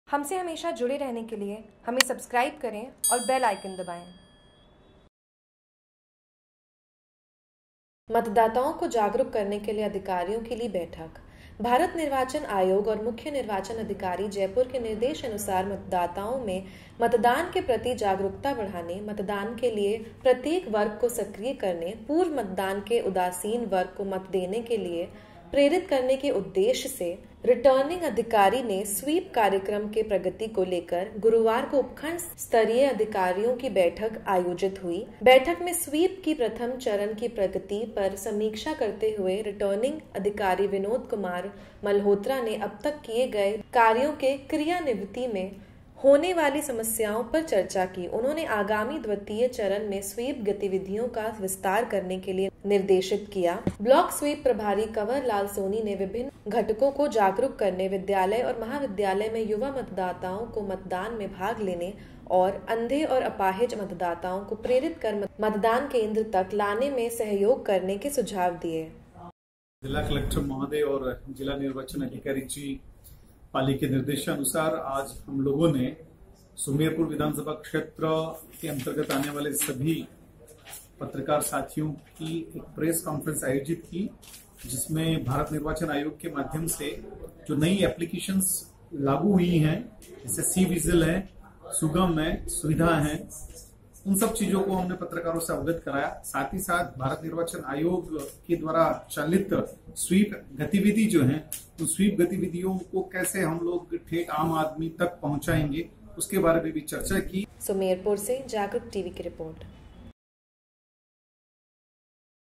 JAGRUK TV : सुमेरपुर : जागरूकता बढ़ाने चुनाव अधिकारियों की बैठक ।
सुमेरपुर : भारत निर्वाचन आयोग व मुख्य निर्वाचन अधिकारी जयपुर के निर्देशानुसार मतदाताओं मे मतदान के प्रति जागरूकता बढाने, मत देने के लिए प्रत्येग वर्ग को सक्रिय करने, पुर्व मतदान के उदासीन वर्ग को मत देने के लिए प्रेरित करने के उद्देश्य से रिर्टनिंग अधिकारी ने स्वीप कार्यक्रम की प्रगति को लेकर गुरूवार को उपखण्ड स्तरीय अधिकारियों की बैठक आयोजित हुई। बैठक में स्वीप के प्रथम चरण की प्रगति पर समीक्षा करते हुए  रिटर्निंग अधिकारी विनोद कुमार मल्होत्रा ने अब तक किए गए कार्यो के क्रियान्विति मे होने वाली समस्याओं पर चर्चा की। उन्होंने आगामी द्वितीय चरण में स्वीप गतिविधियों का विस्तार करने के लिए निर्देशित किया। ब्लॉक स्वीप प्रभारी कंवरलाल सोनी ने विभिन्न घटको को जागरूक करने, विद्यालय एवं महाविद्यालय में युवा मतदाताओं को मतदान में भाग लेने तथा अंधे व अपाहिज मतदाताओं को प्रेरित कर मतदान केन्द्र तक लाने में सहयोग करने के सुझाव दिए ।